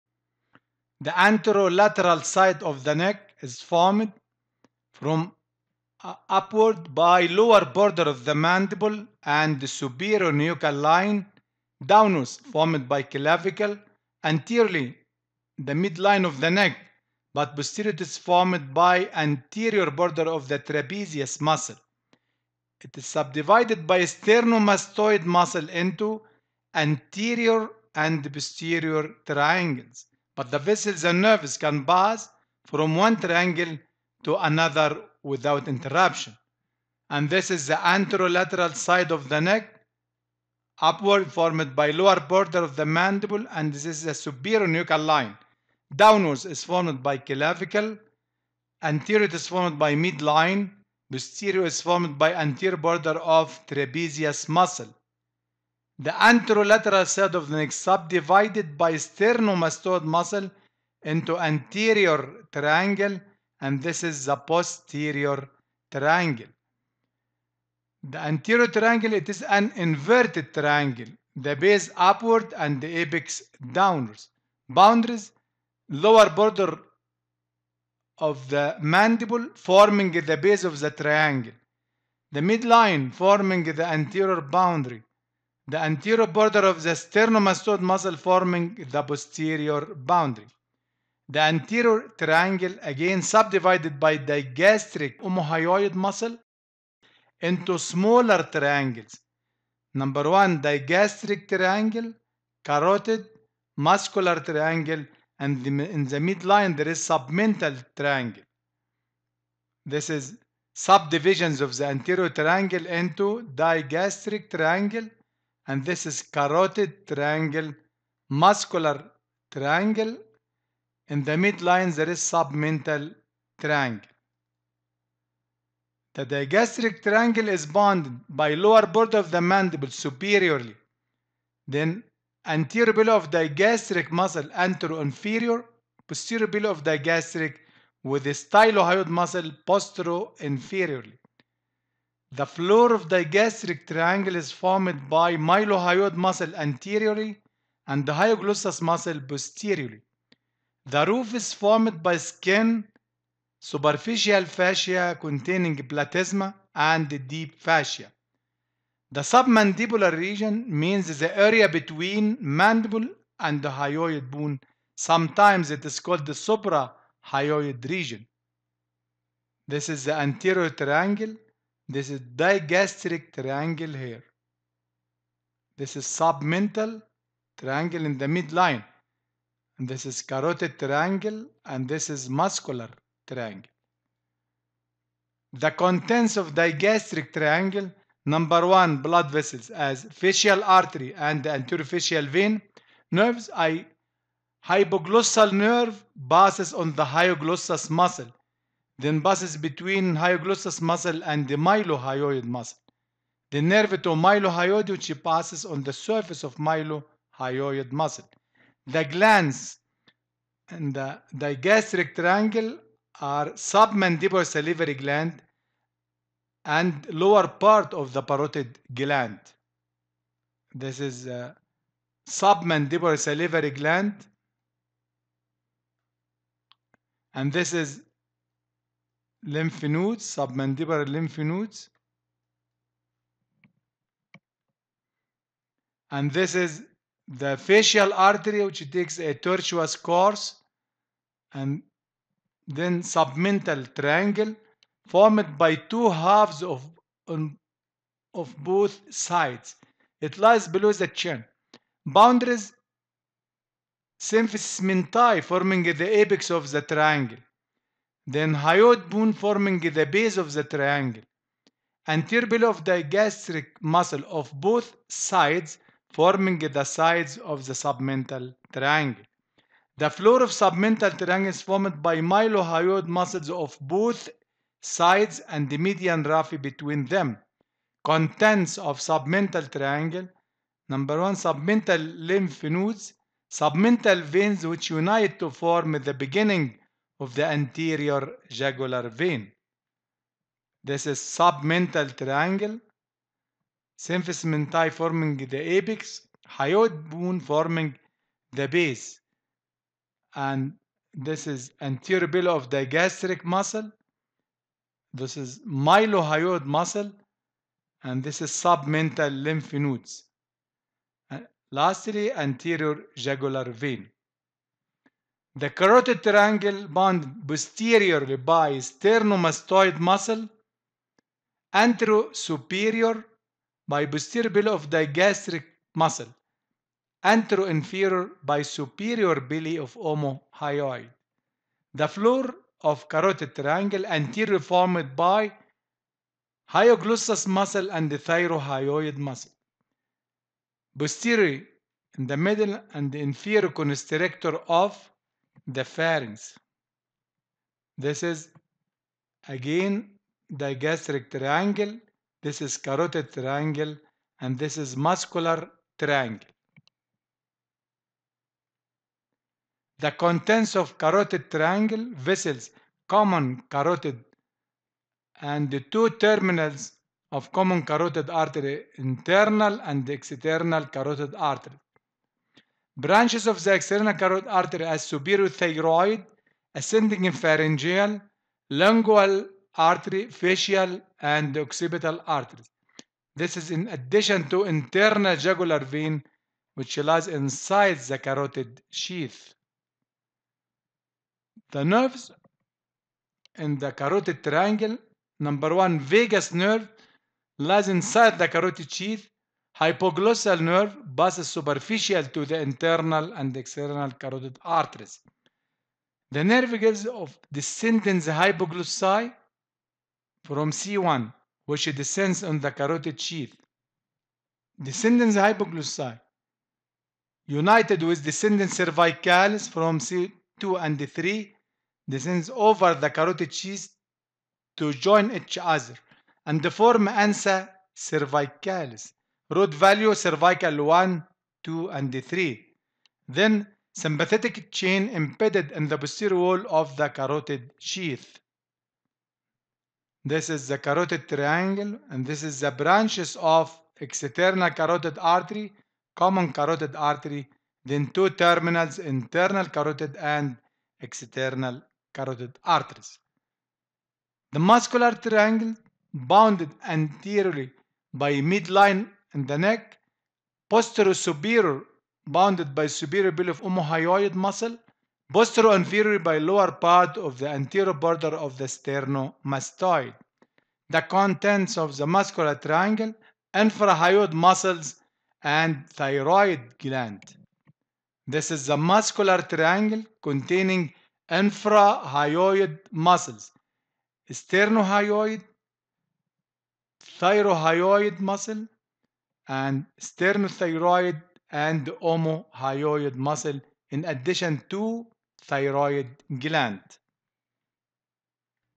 Anatomy 2 | U1 - L8 | Anterior triangle of the neck
Unit 1 : Head and neck
Lecture 8 : Anterior triangle of the neck
- Antero-lateral side of the neck
- Subdivision of the anterior triangle
- Contents of the anterior triangle